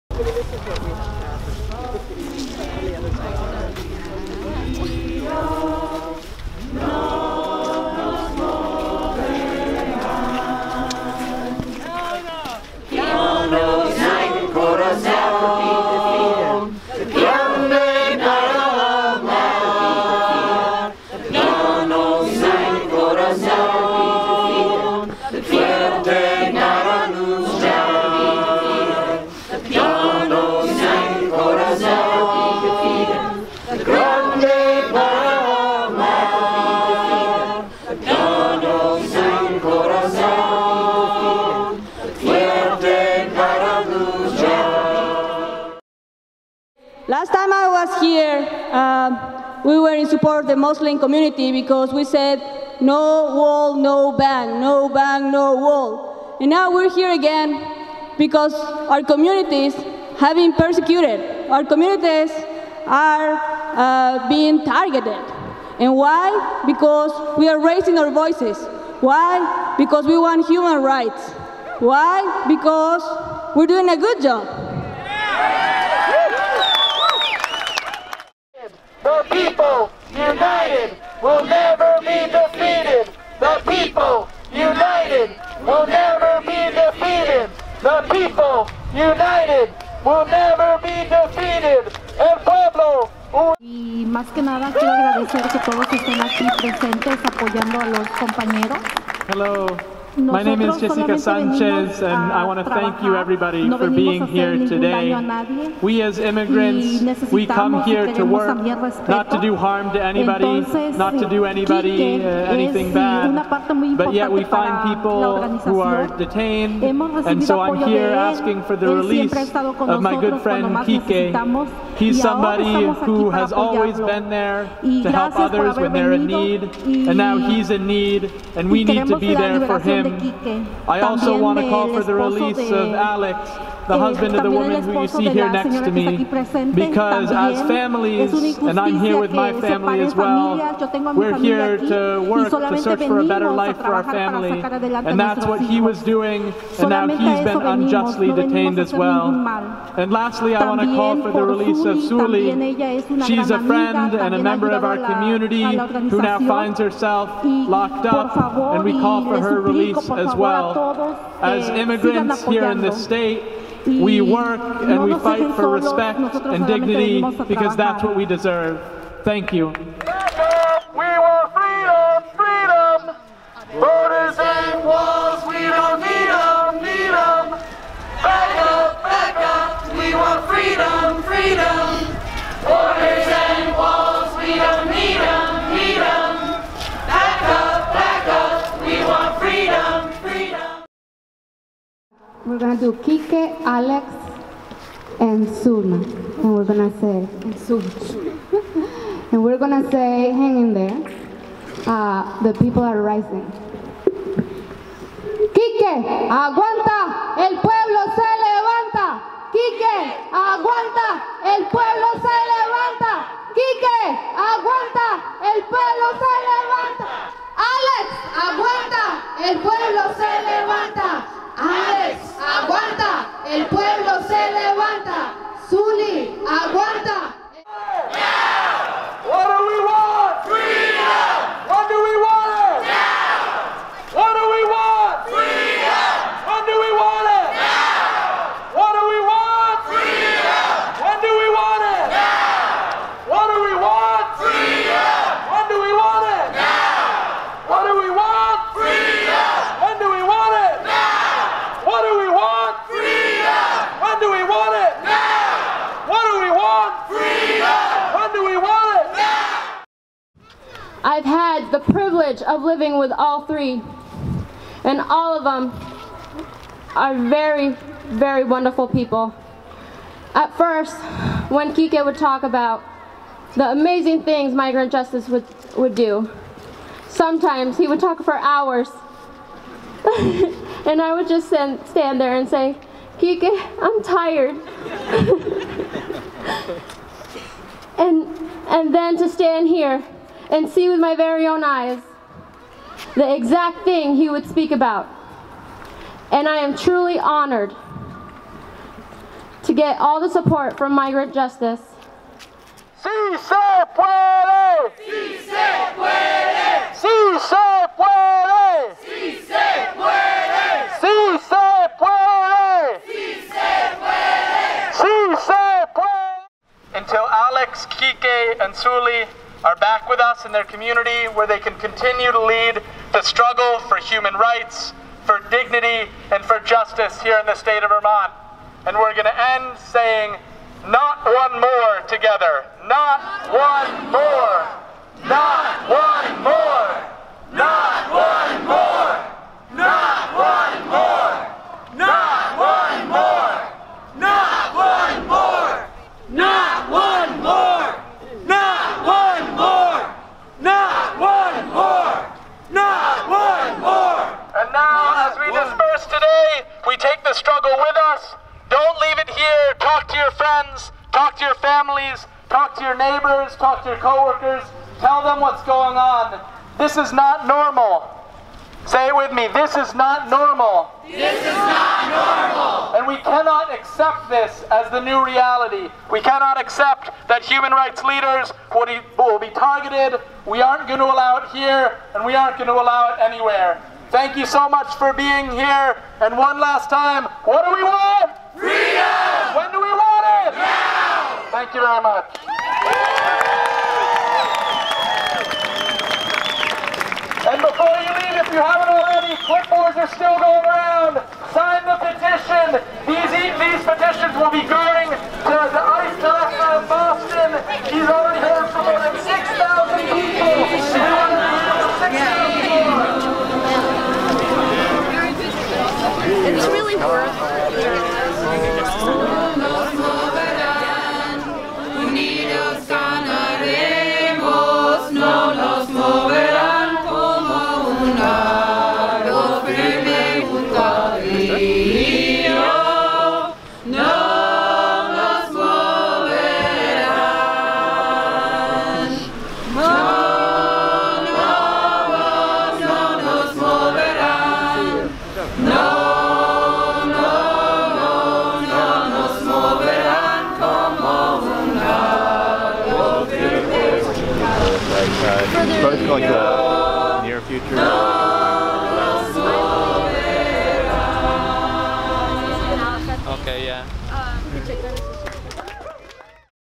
Kike, Zully, Alex! Aguanta! El Pueblo Se Levanta! - Migrant Justice March on Montpelier
Kike, Zully, Alex! Aguanta! El Pueblo Se Levanta! (Kike, Zully, Alex! Hold On! The People Are Rising Up!)

Friends and allies march on Montpelier, Vermont and speak out at the Vermont State House in support of arrested Migrant Justice activists, Kike, Zully, and Alex.

March 2017.

Produced by Christopher Wiersema for Channel 17/Town Meeting TV in Burlington, Vermont.